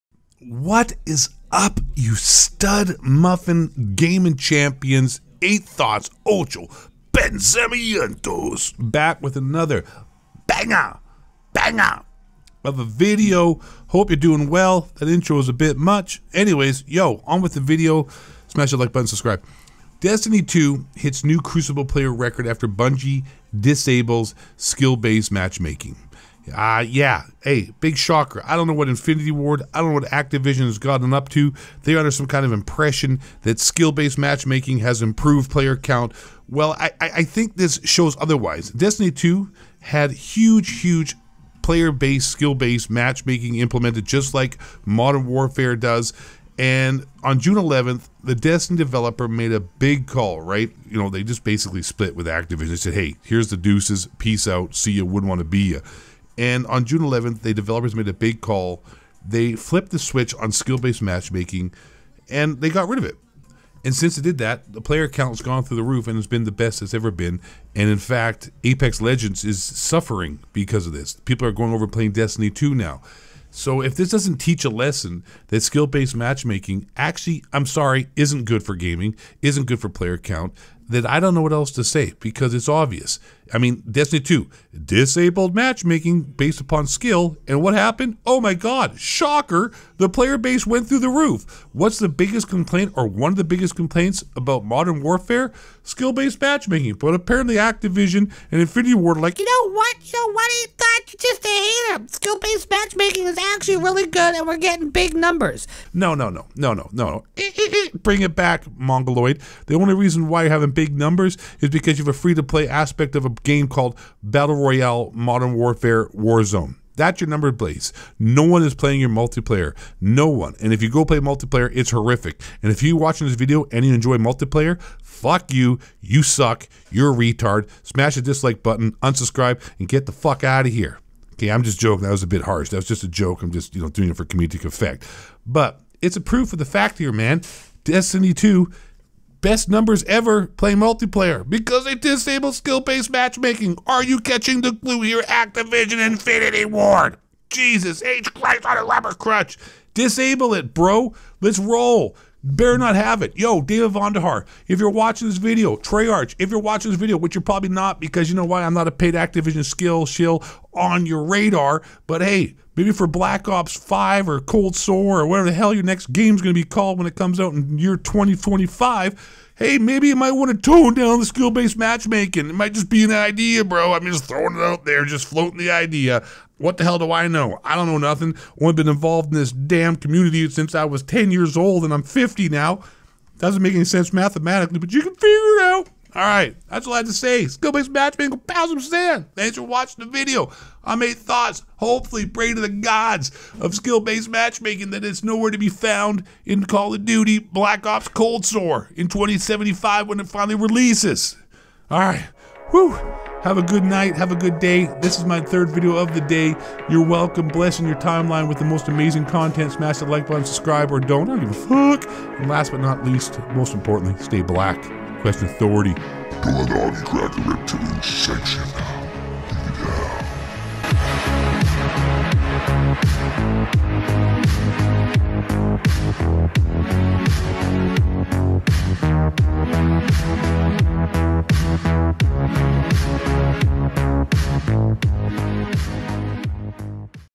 Destiny Disabled SBMM, Record High Player Count - Hope COD 2020 Does the Same (Jul 3, 2020)
Originally posted on the 3rd of July 2020

Bungie proved with Destiny that removing SBMM is better for multiplayer.